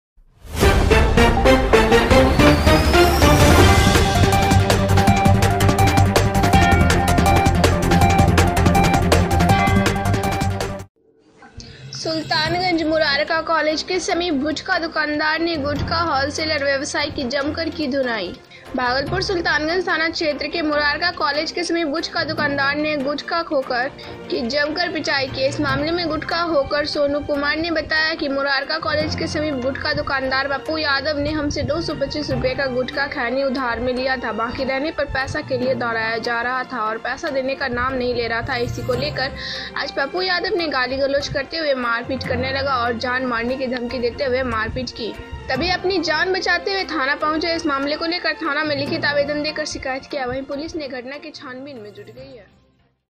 सुल्तानगंज मुरारका कॉलेज के समीप गुटखा दुकानदार ने गुटखा होलसेलर...|jankari junction
सुल्तानगंज मुरारका कॉलेज के समीप गुटखा दुकानदार ने गुटखा होलसेलर व्यबसायी की जमकर किए धुनाई।
भागलपुर सुल्तानगंज थाना क्षेत्र के मुरारका कॉलेज के समीप गुटखा दुकानदार ने गुटखा होकर की जमकर किए पिटाई। इस मामले में गुटखा होकर सोनू कुमार ने बताया कि मुरारका कॉलेज के समीप गुटखा दुकानदार पप्पू यादव ने हम से 225 रुपया का गुटखा खैनी उधार में लिया था । बाकी रहने पर पैसा के लिए दौड़ाया जा रहा था । और पैसा देने का नाम नही ले रहा था इसी को लेकर आज पप्पू यादव ने गली गलौज करते हुए मारपीट करने लगा।और जान मारने की धमकी देते हुए मारपीट किए। तभी अपनी जान बचाते हुए थाना पहुँचे। इस मामले को लेकर थाना में लिखित आवेदन देकर शिकायत किया है।वहीं पुलिस ने घटना की छानबिन में जुट गई है।